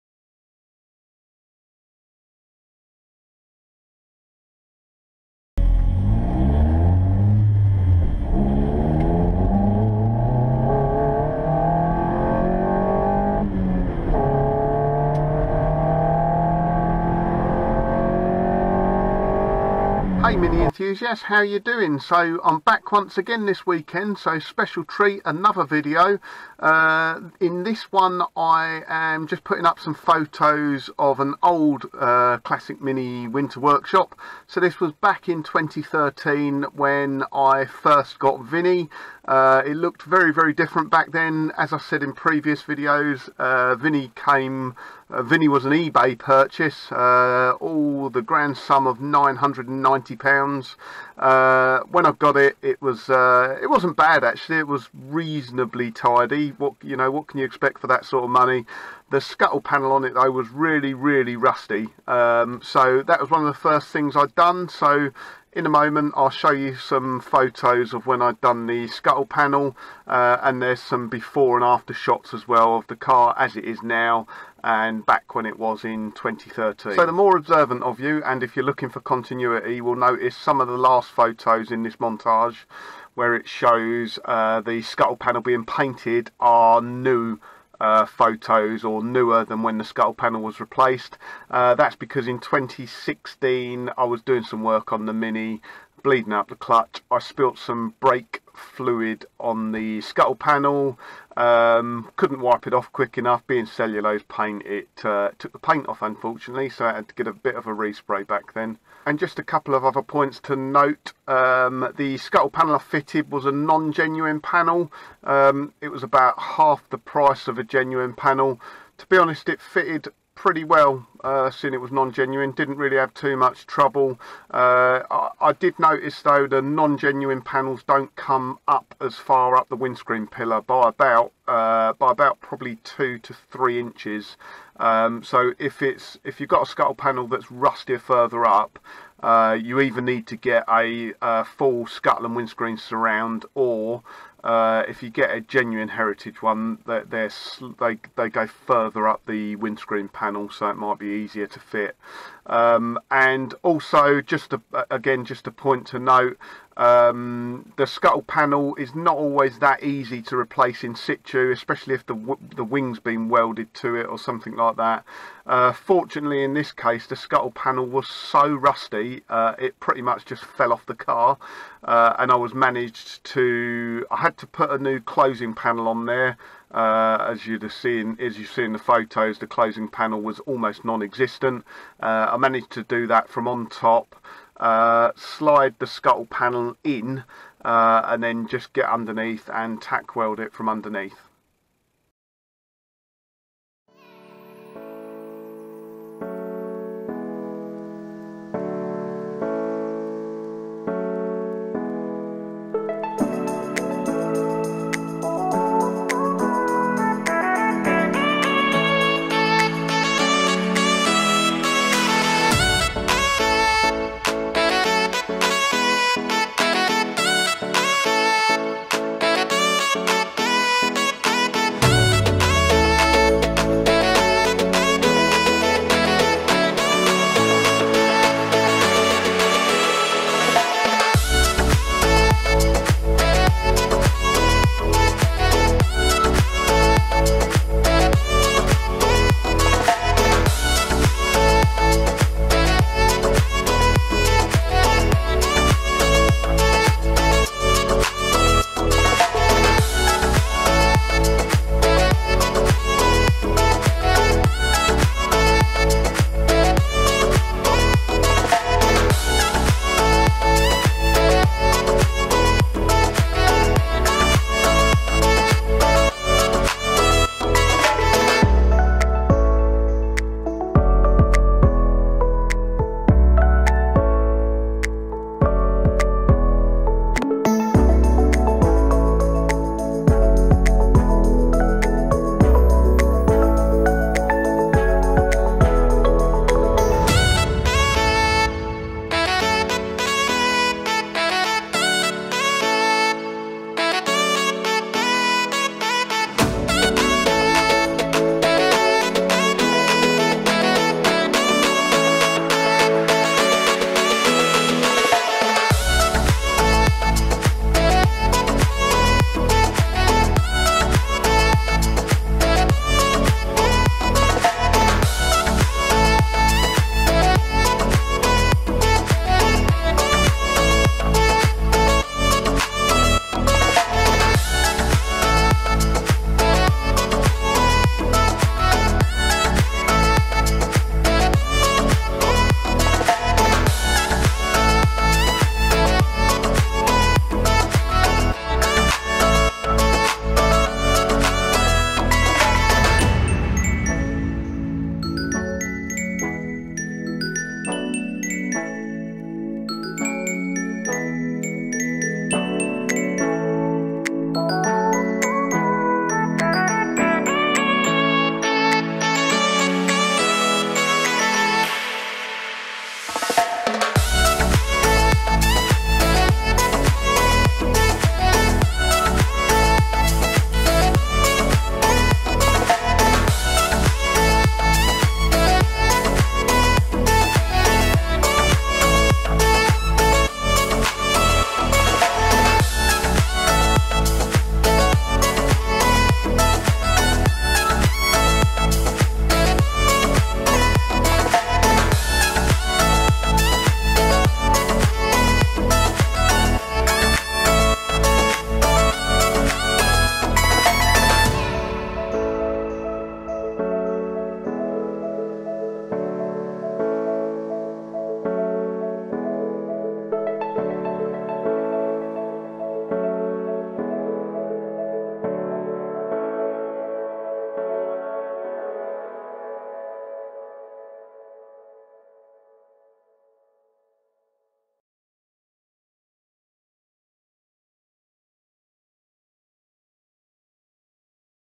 Classic Mini Scuttle Panel Relplacement
In 2013 I replaced the windscreen scuttle on my Mini. The old scuttle was so rusty it didn't take much to remove. With some careful cutting I was able to do the job without damaging the wings.